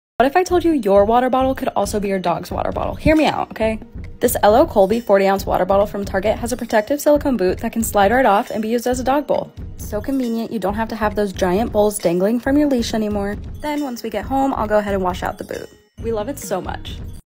Water Bottle for On-The-Go Pet Owners
The Colby 40oz Stainless Steel Water Bottle has a silicone boot that can be used as an on-the-go dog bowl.